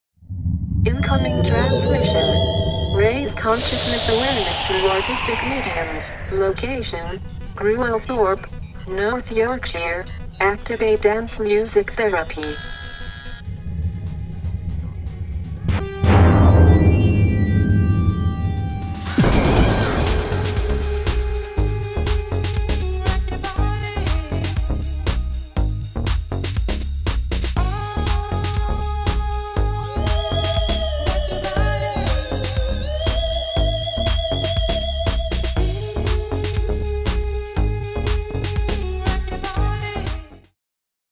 Dance Music Therapy @ Limetree Festival 2011 Activate!!
Short preview viral for the all new Dance Music Therapy Village at this years Limetree Festival 2011!

Watch this space for 3 further videos from Ugroove TV and Future Arts showcasing the fantastic story of 'Every Road Leeds to Chicago' featuring some of our top guest DJ's from this years Limetree Festival!